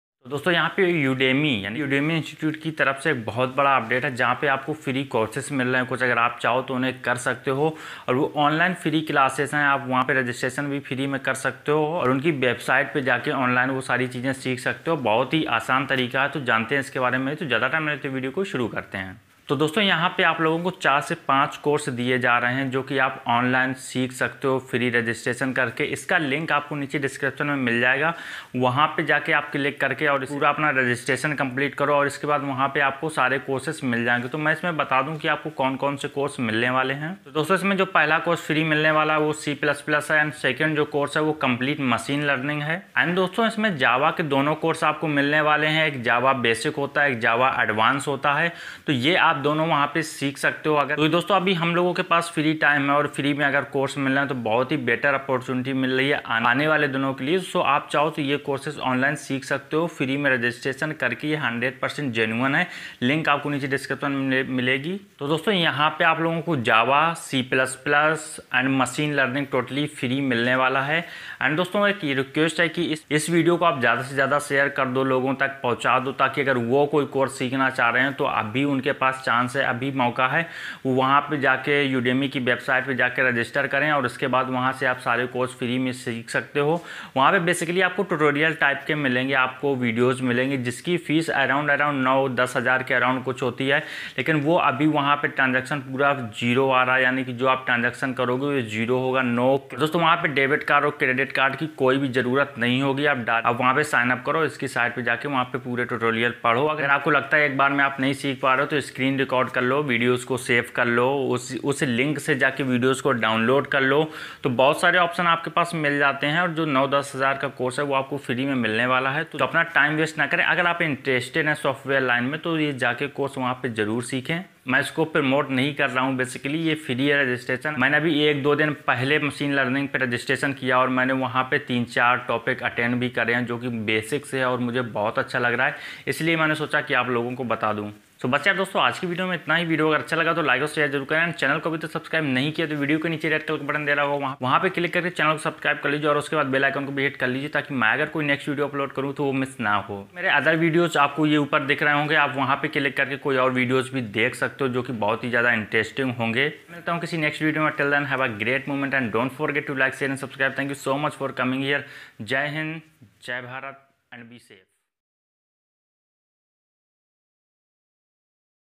जल्दी करो अब सारे IT के Online Courses बिल्कुल फ्री है ( IT & Software ) ? Free online course join
Hi Friends .
Maine is video me btaya h udemy website ke bare me jo abhi online free kuch course de rahi sayd aap interest ho...

Udemy: Free Couses 100%

[FREE] Java Tutorial for Complete Beginners | Full 16 Hr couse

Share  group link with your family members and friends

Thanks.